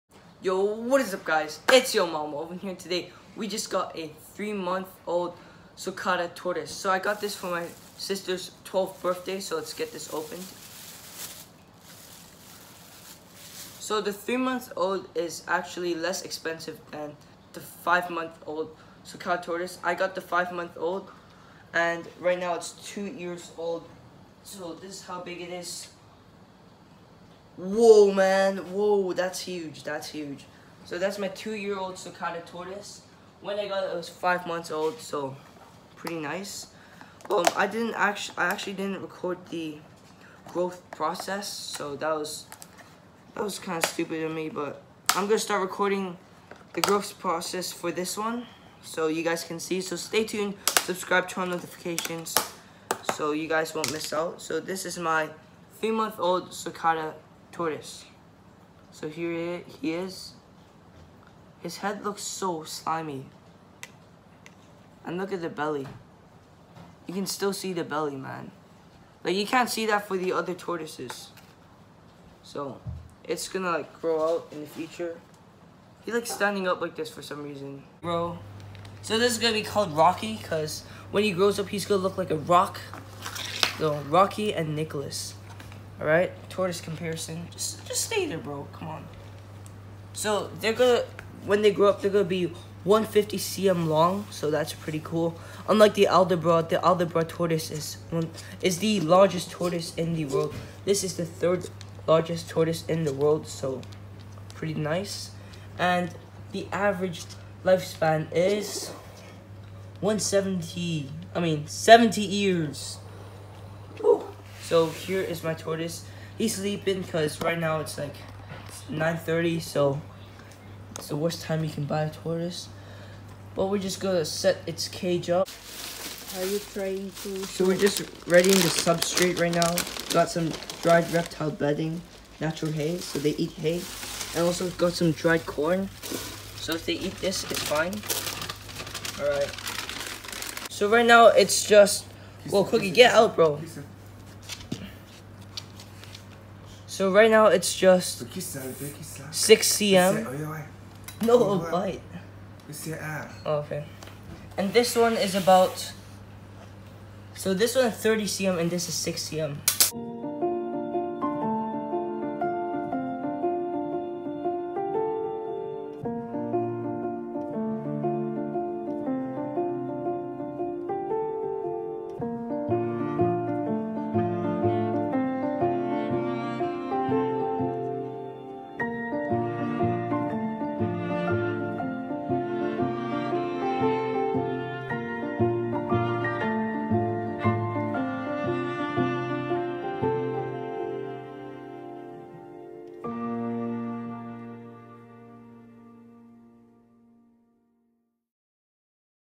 UNBOXING AN EXOTIC BABY SULCATA TORTOISE
Today I got a cute little tortoise named ROCKY that in my opinion would be great pets for beginner tortoise enthusiasts.